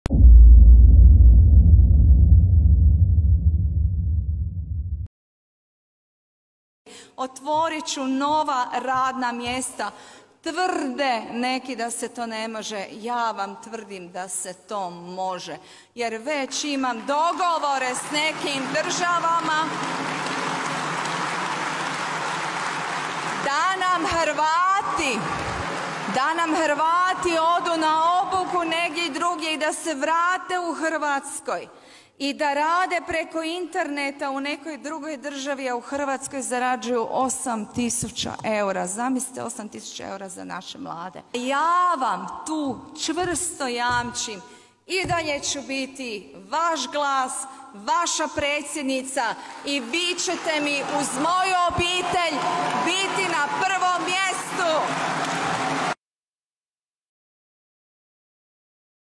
Plaća od 8000 eura - Kolindino predizborno obećanje
Ako je moguća plaća od 8000 eura za legalni rad na internetu, zašto je Kolinda Grabar-Kitarović čekala cijeli jedan predsjednički mandat kako bi takvo što omogućila? Čini se da predizborna obećanja postaju sve luđa, a zašto je to tako vi procijenite sami.